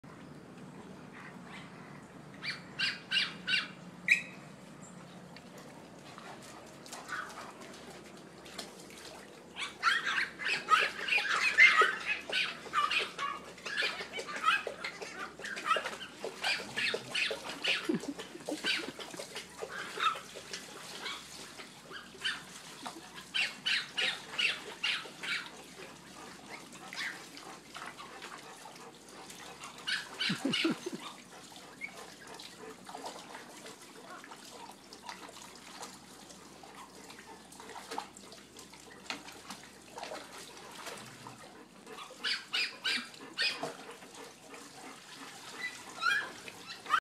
Care for a swim? Water Wolfhounds
I thought Irish Wolfhounds do not like the water???